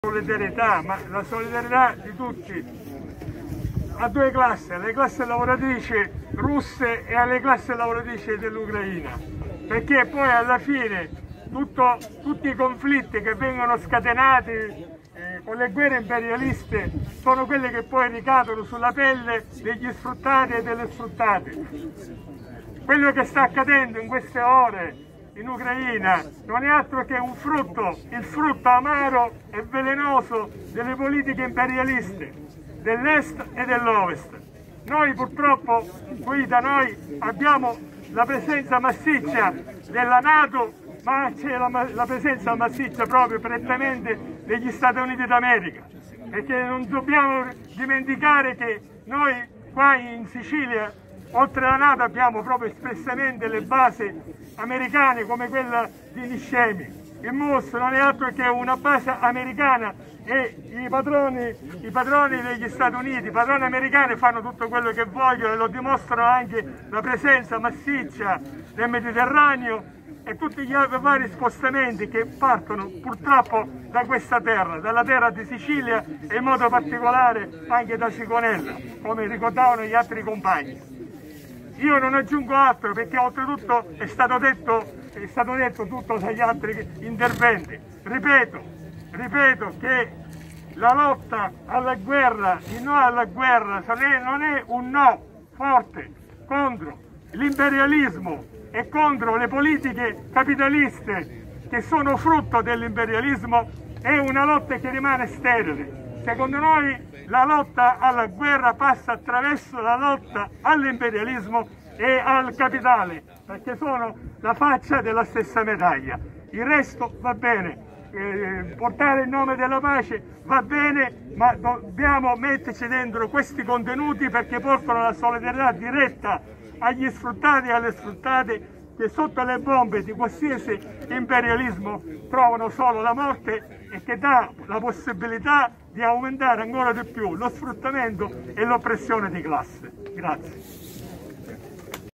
USB al porto di Catania contro la guerra imperialista. Intervento di Orazio Vasta
Oggi, 27 febbraio 2022, di mattina, al PORTO DI CATANIA, molo 4, si è svolto un PRESIDIO "CONTRO LA MILITARIZZAZIONE E LA GUERRA IMPERIALISTA", manifestazione indetta dal Movimento No Muos e dalla Rete Antirazzista Catania.
Presente la Federazione del Sociale USB Catania intervenuta al megafono con il compagno Orazio Vasta, responsabile provinciale ASIA USB Catania.

➖NO GUERRA
➖NO NATO
➖NO EU